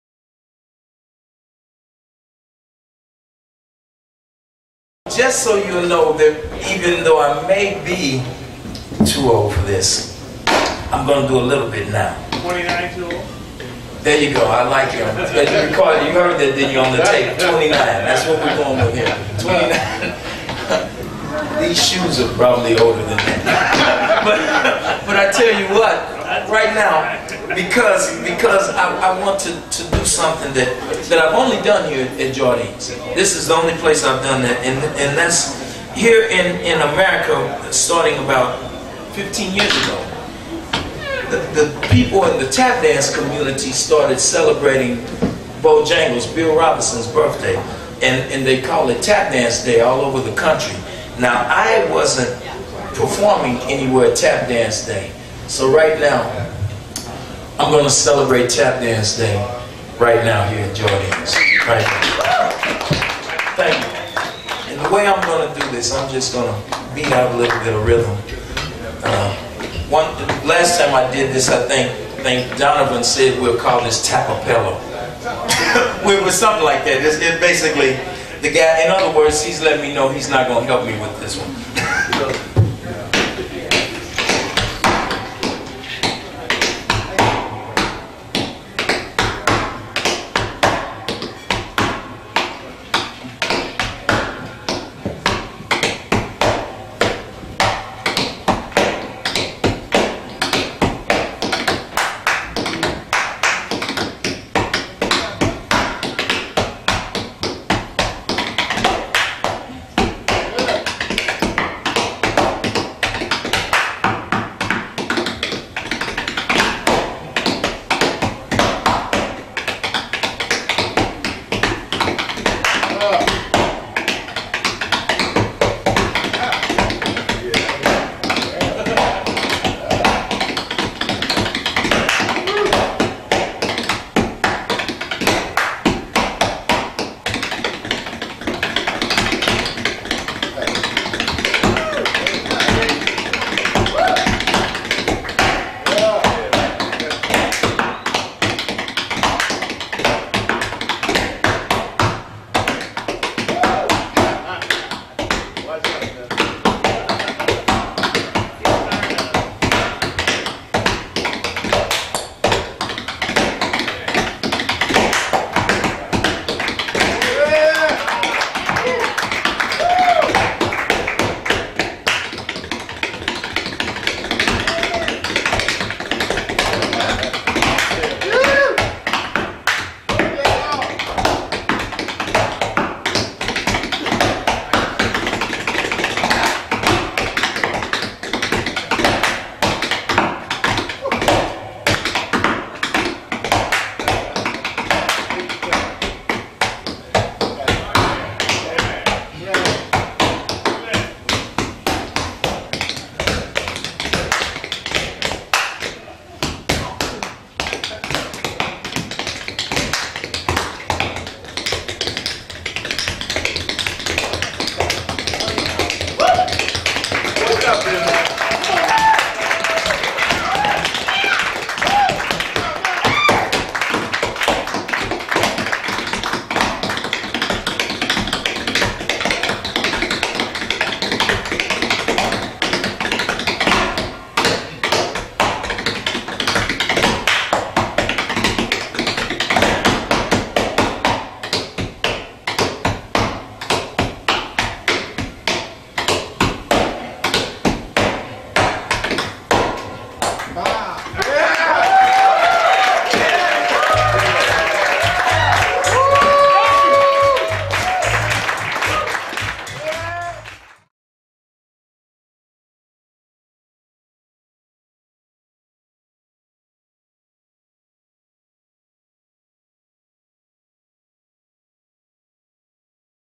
Lonnie McFadden Tap Dancing at Jardines on the Plaza in Kansas City
Lonnie McFadden doing a short national Tap Dance day routine at Jardines on the Plaza in Kansas City.